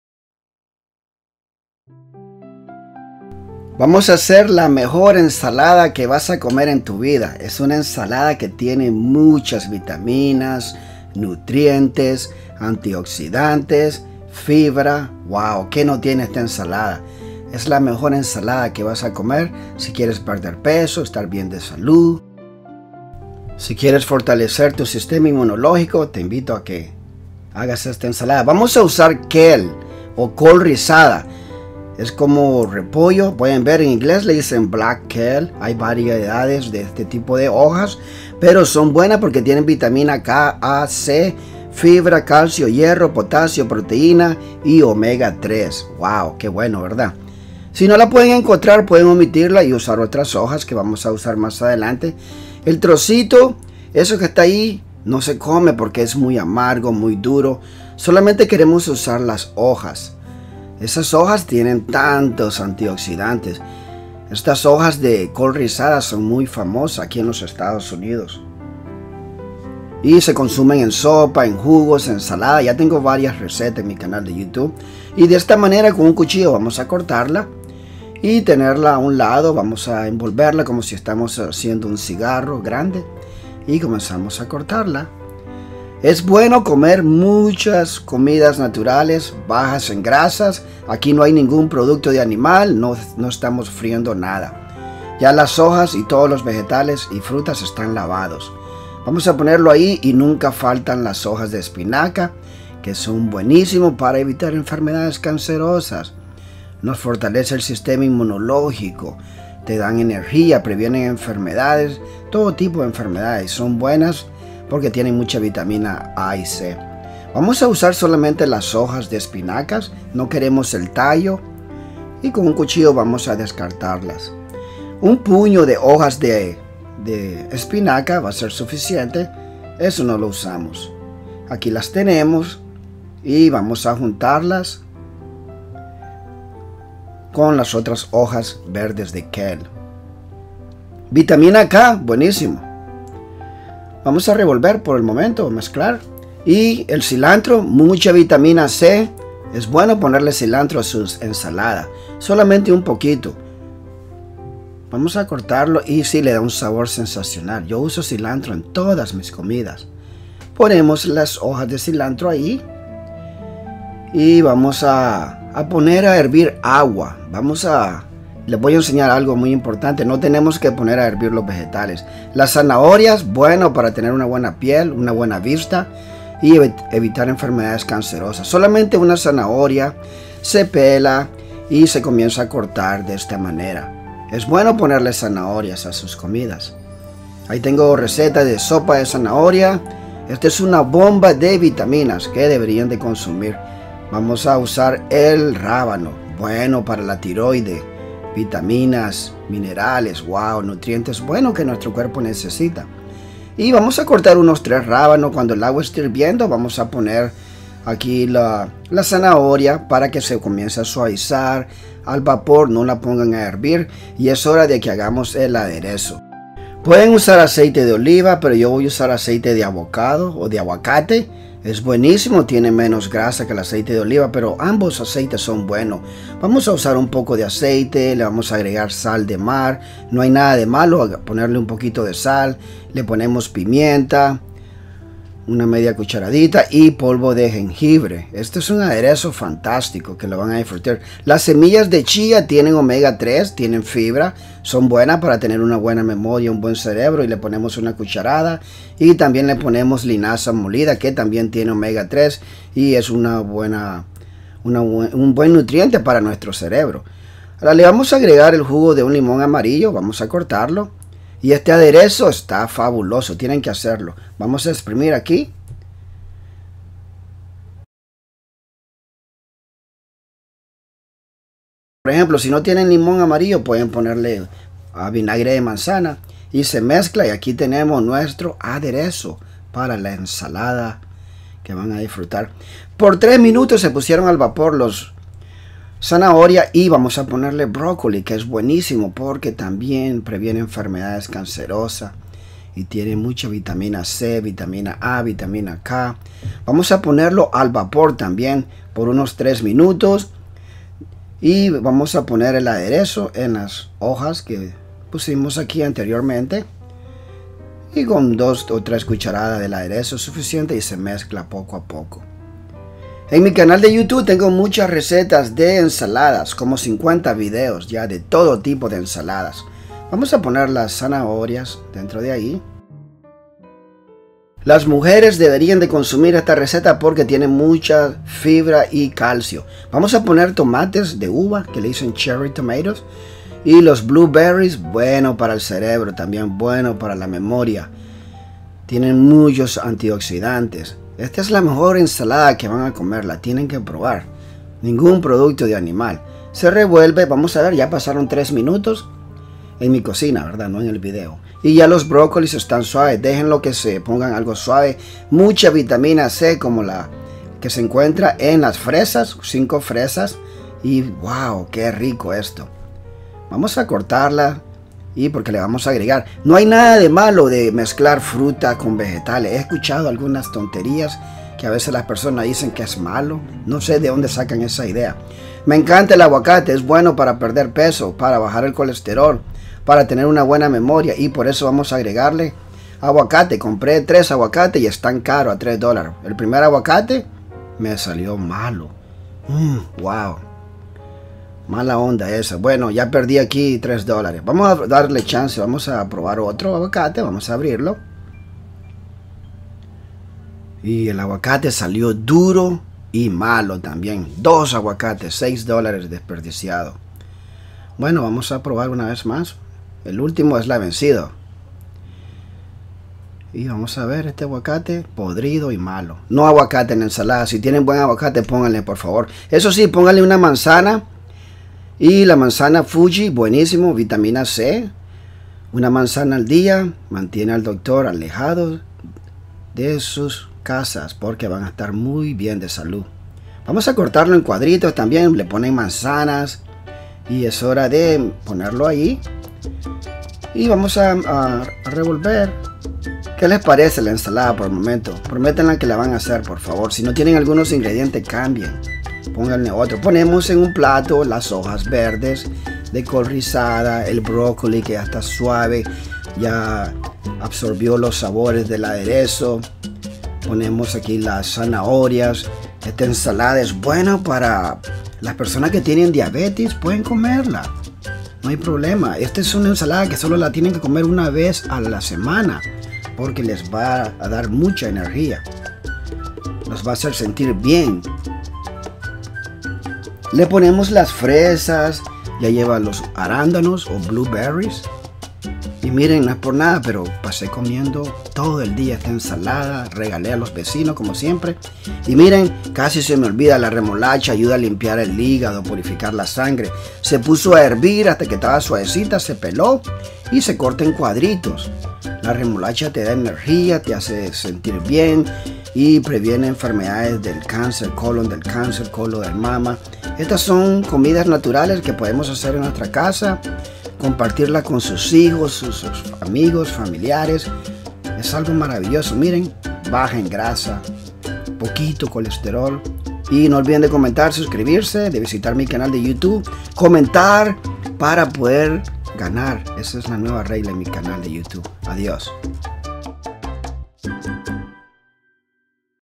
La mejor ensalada que vas a comer en tu vida.
Una super saludable y deliciosa ensalada verde vegana  con kale, o col rizada, brócoli, fresas,  zanahorias, arándanos, hojas de espinacas, chía, remolachas, semillas de linaza, tomates, aguacates y mucho mas. Esta ensalada es ideal para prevenir enfermedades cancerosas, fortalecer el sistema inmunológico, perder peso ,y te da mucha energia.